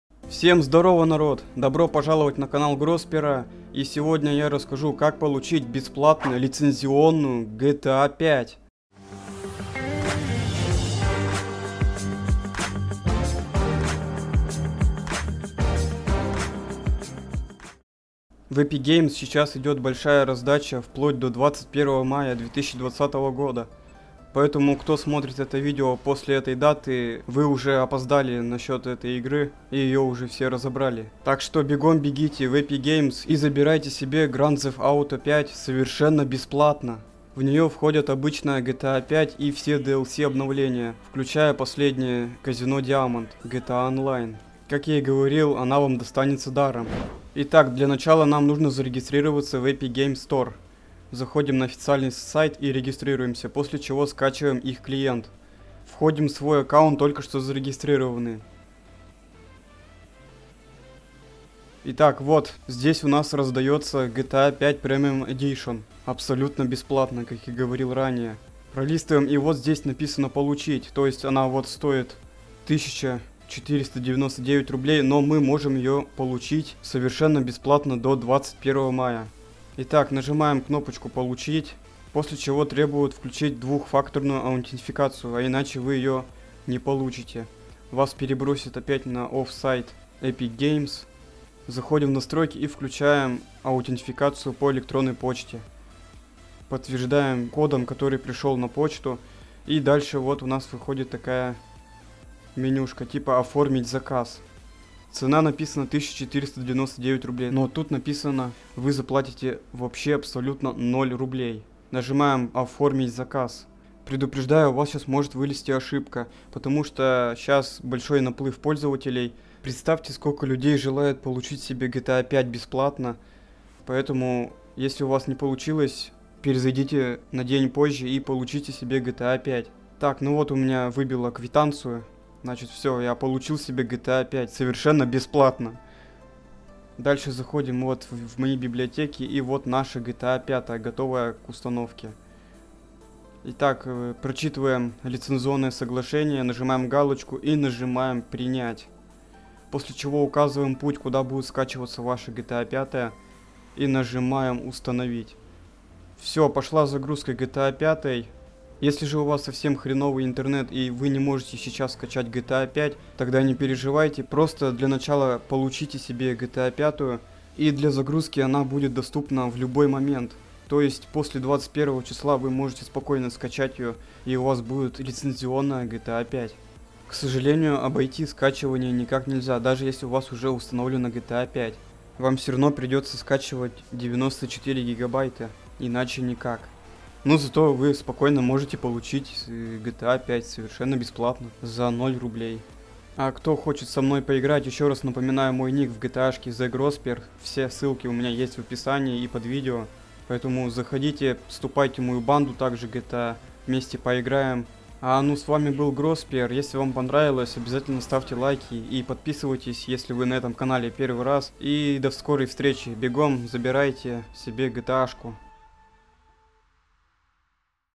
Как Получить Лицензионную GTA 5 БЕСПЛАТНО? Раздача игр в Epic Games
Всем привет, друзья! Добро Пожаловать на канал Грозпера!
Кто хочет получить себе ГТА 5 за 0 руб? Тогда быстрей бегите в сервис Epic Games Store и забирайте GTA 5!
P.S: Раздача закончилась'(
ϟ Нажми ЕЩЁ чтобы увидеть все описание ϟ